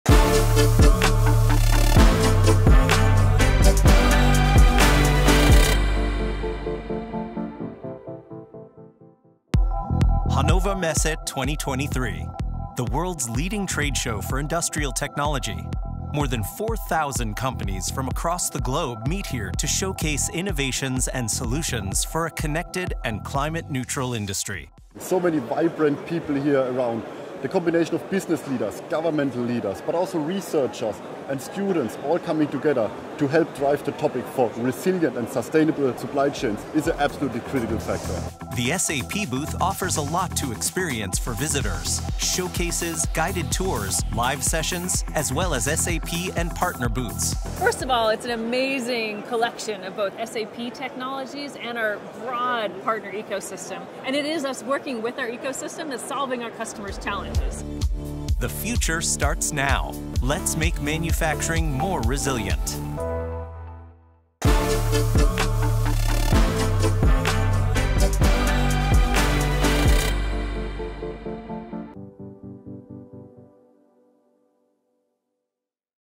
Highlight moments from Hannover Messe HM2023 with SAP
A 'sizzle reel' here of all the highlights from hannovermesse with sap !
With the world of industry changing faster than ever, this event has always been at the forefront of that change with the current trajectory of innovation and integration of emergent technologies for a more resilient, networked and carbon neutral industry. Key trends include energy, artificial intelligence and automation alongside IT Solutions, metaverse and R&D.